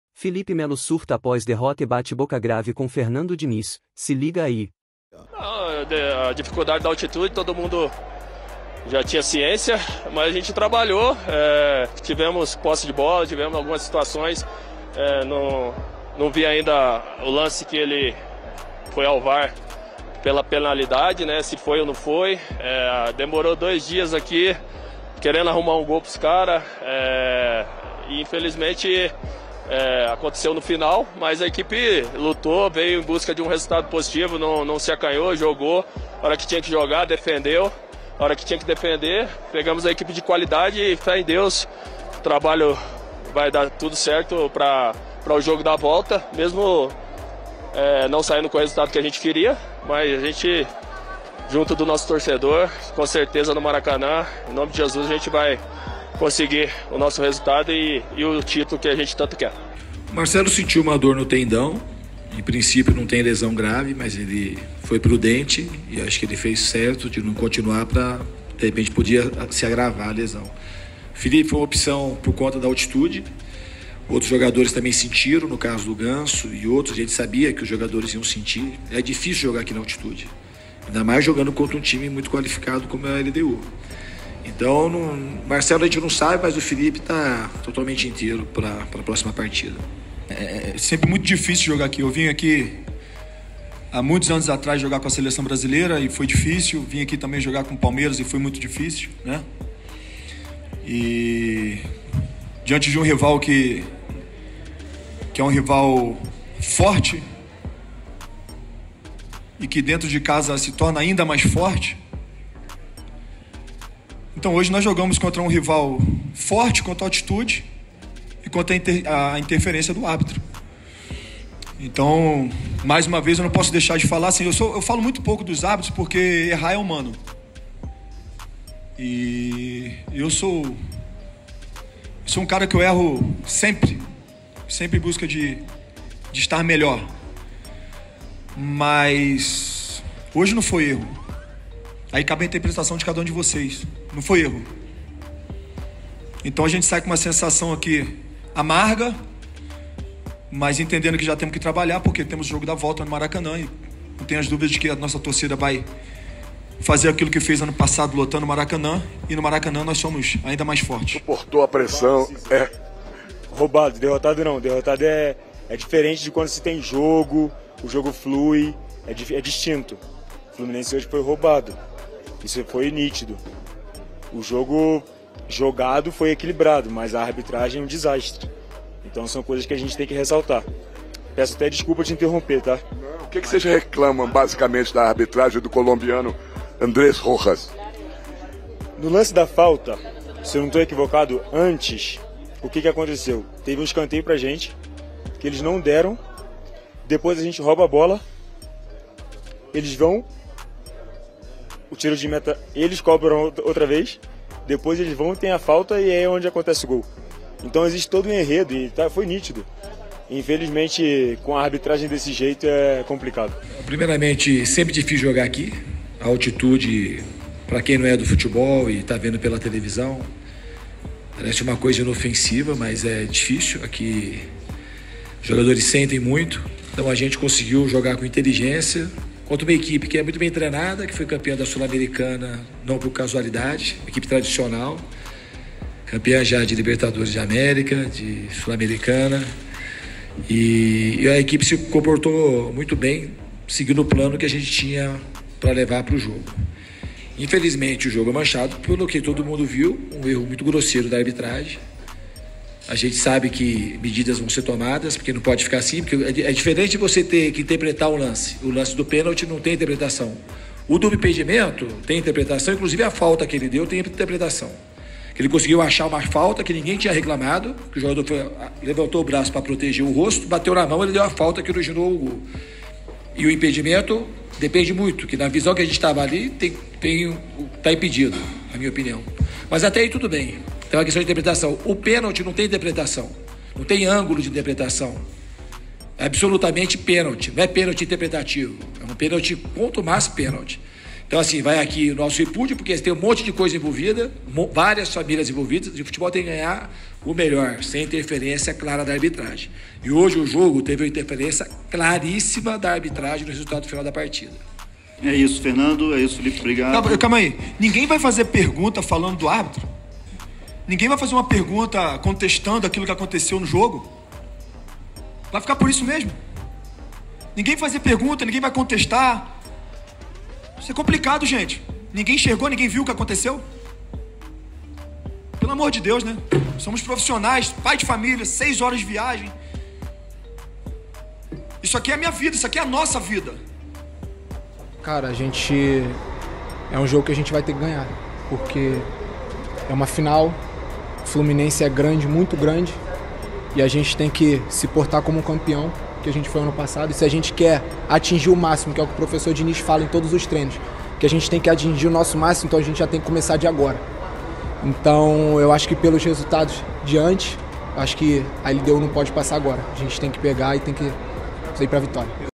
TRETA! FELIPE MELO DETONA EM COLETIVA APÓS DERROTA DO FLUMINENSE!
Após a derrota avassaladora, Felipe Melo, transtornado, protagonizou um bate-boca fervoroso com Diniz. A tensão pairava no ar enquanto palavras afiadas eram trocadas, ecoando pelo vestiário. Melo, inflamado pela frustração, expressou sua indignação em alto e bom som, desafiando a liderança de Diniz e expondo as fissuras na equipe. O ambiente se tornou um campo de batalha verbal, onde cada acusação lançada reverberava como um trovão, deixando todos atônitos diante do confronto explosivo entre dois pilares da equipe.